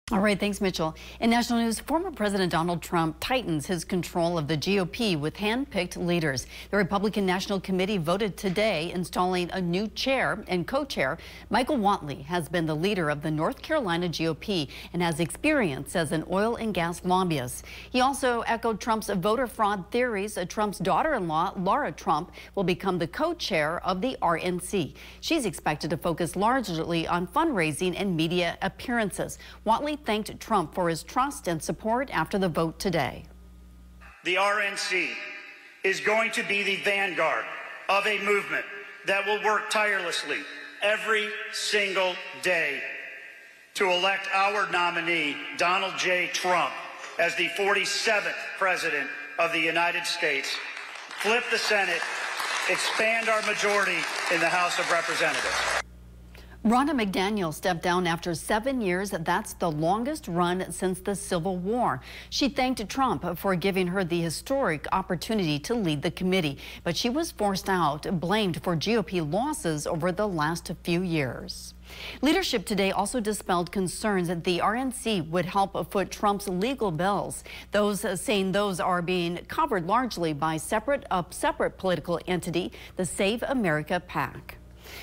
Trump's chosen leadership team assumes control of Republican National Committee
The Republican National Committee voted Friday to install Donald Trump's handpicked leadership team, completing his takeover of the national party as the former president closes in on a third straight presidential nomination.

Michael Whatley, a North Carolina Republican who has echoed Trump's false theories of voter fraud, was elected the party's new national chairman in a vote Friday morning in Houston. Lara Trump, the former president's daughter-in-law, was voted in as co-chair.

Trump's team is promising not to use the RNC to pay his mounting personal legal bills. But Trump and his lieutenants will have firm control of the party's political and fundraising machinery with limited, if any, internal pushback.